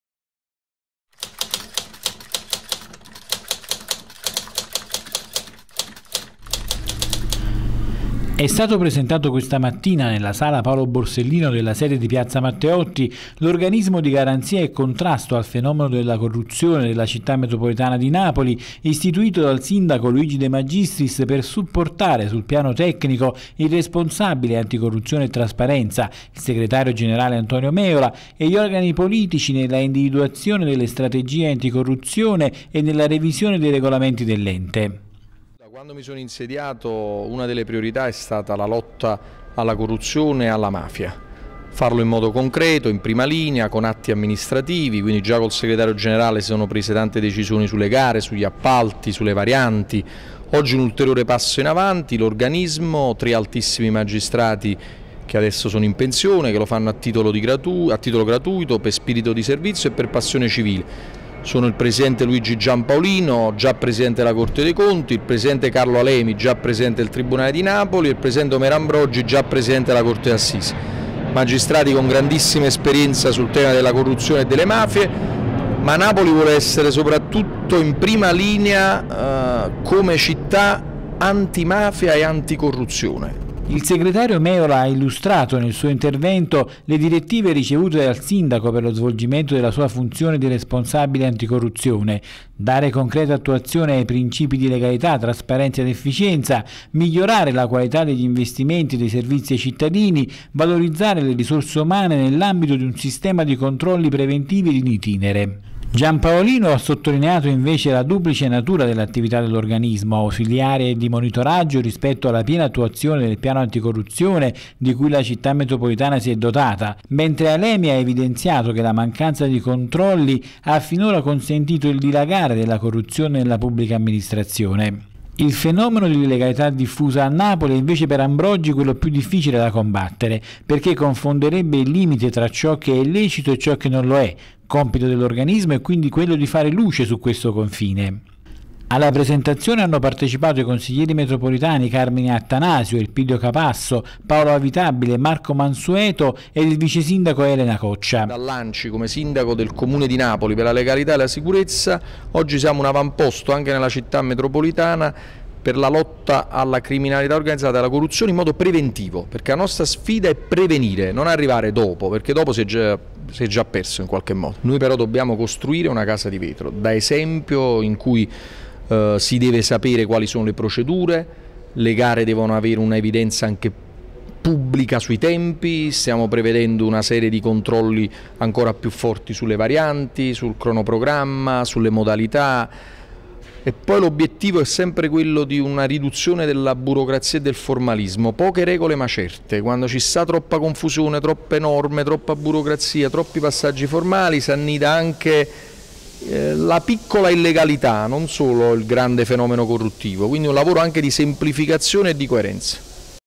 Città metropolitana, istituito l’organismo anticorruzione.
È stato presentato questa mattina, nella sala “Paolo Borsellino” della sede di piazza Matteotti, l'organismo di garanzia e contrasto al fenomeno della corruzione della Città Metropolitana di Napoli istituito dal Sindaco Luigi de Magistris per supportare sul piano tecnico il Responsabile anticorruzione e trasparenza - il Segretario generale Antonio Meola - e gli organi politici nella individuazione delle strategie anticorruzione e nella revisione dei regolamenti dell'Ente. Compongono la struttura Luigi Giampaolino, già Presidente della Corte dei Conti, Carlo Alemi, già Presidente del Tribunale di Napoli, e Omero Ambrogi, già Presidente della III Corte d'Assise d'Appello di Napoli, che lavoreranno a titolo gratuito.